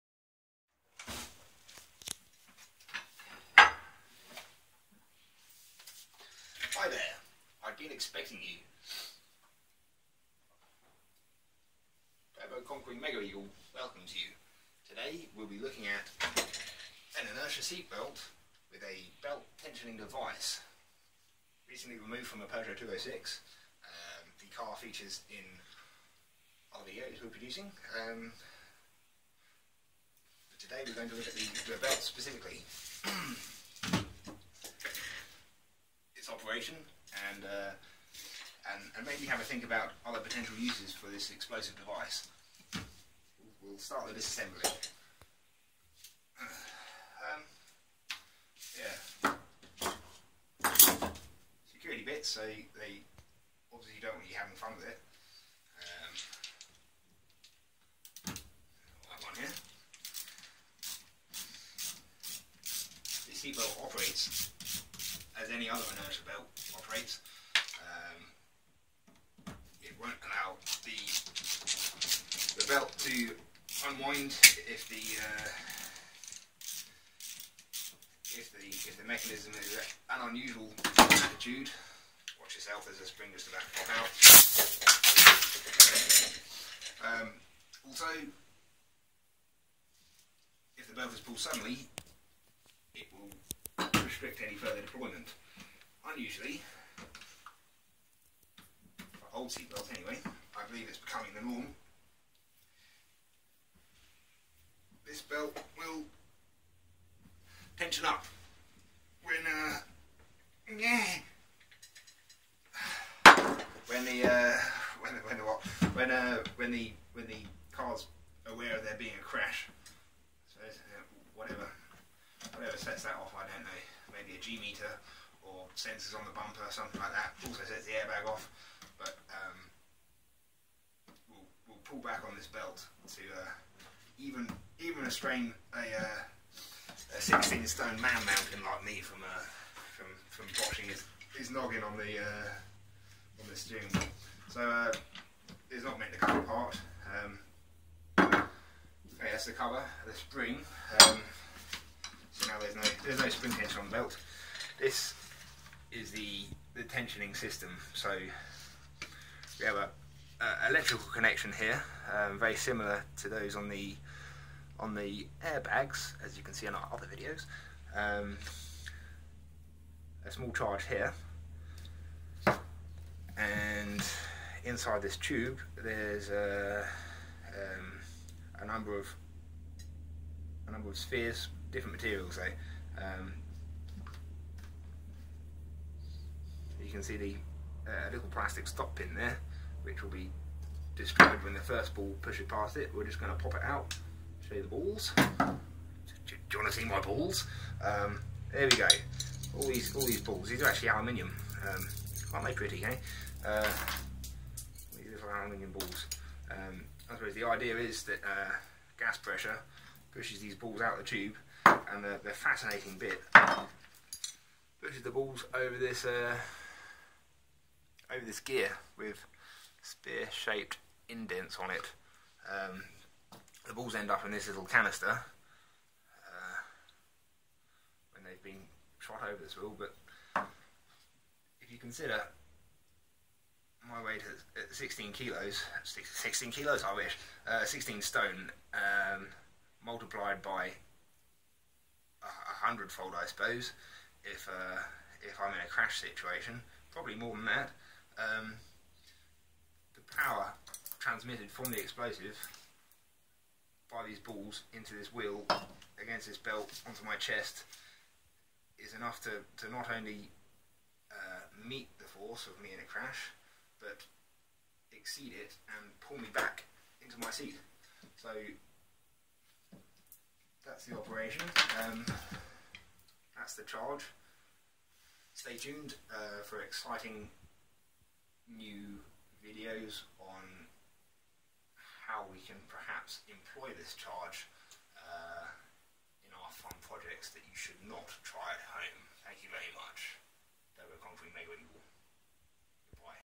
Inertia belt dismantling for next project
In this video we disassemble a car inertia seat belt. This is so we can remove the explosive charge, which we will be using in our next project.